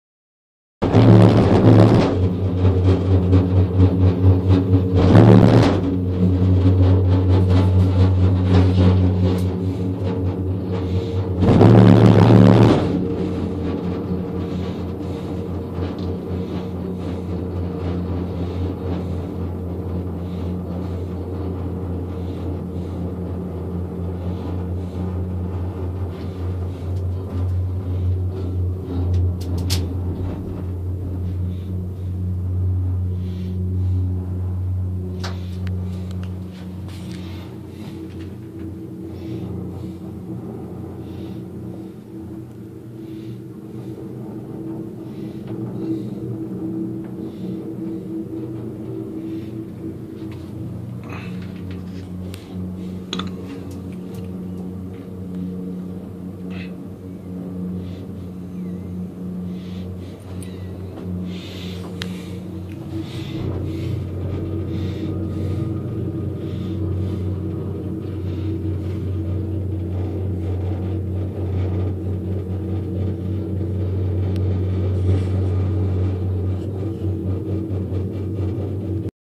Gondola Ride in Park city
I rode the Gondola in Park City Utah during a conference